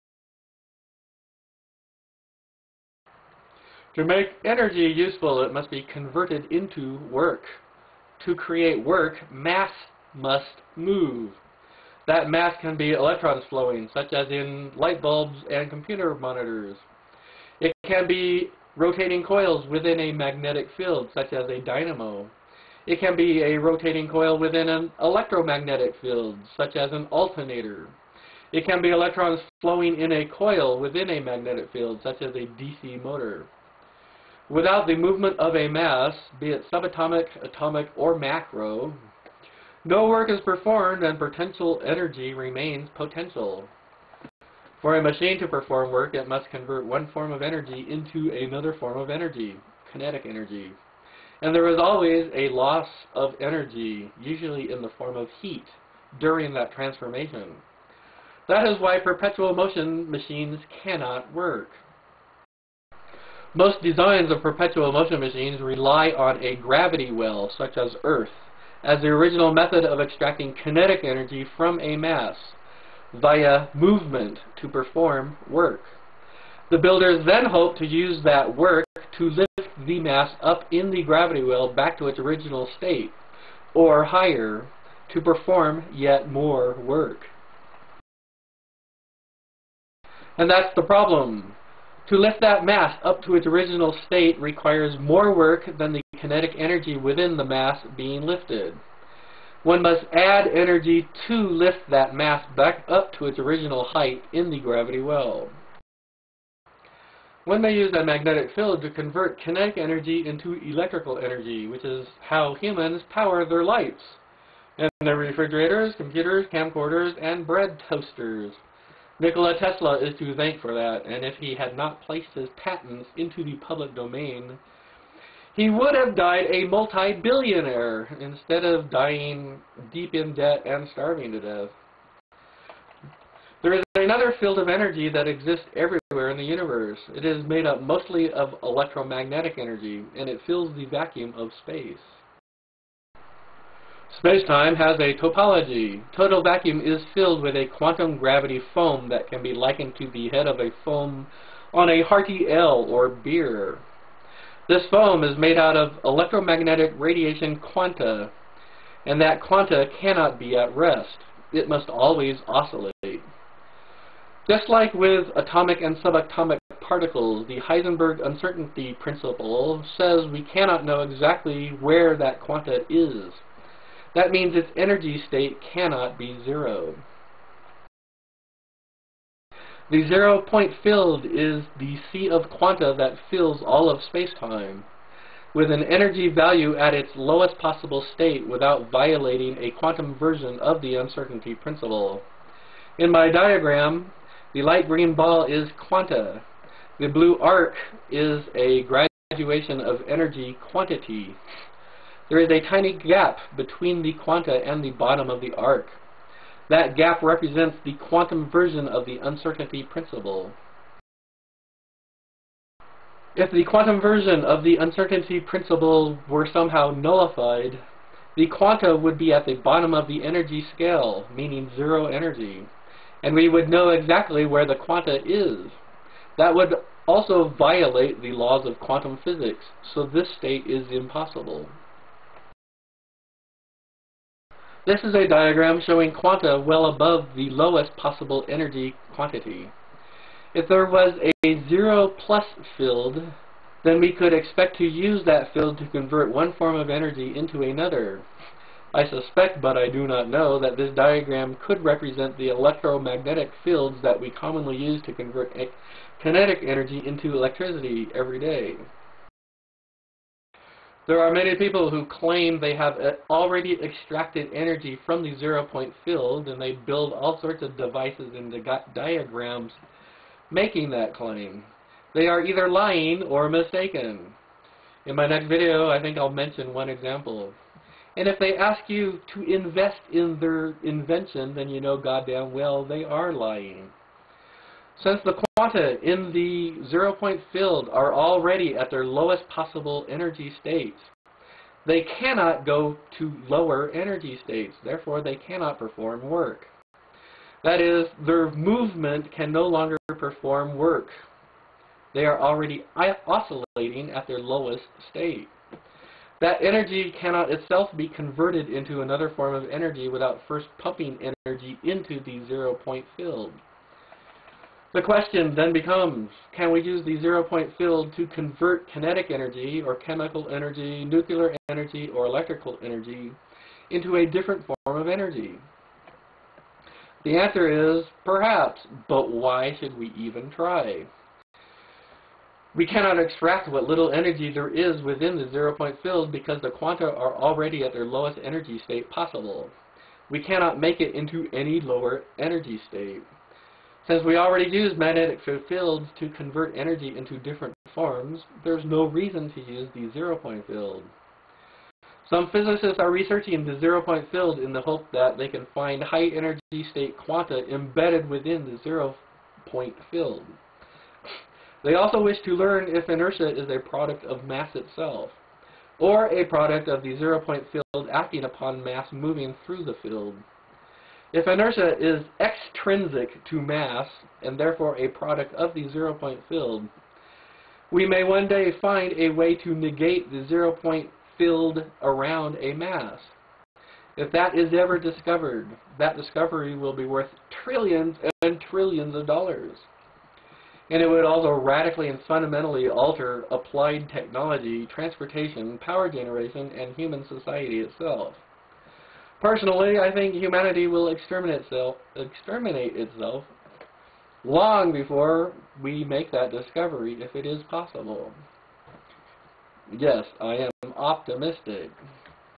Zero Point Field; Free Energy; Perpetual Motion
Getting "free energy" from the Zero Point Field is currently impossible and will probably always be: anyone who says otherwise is either lying or mistaken.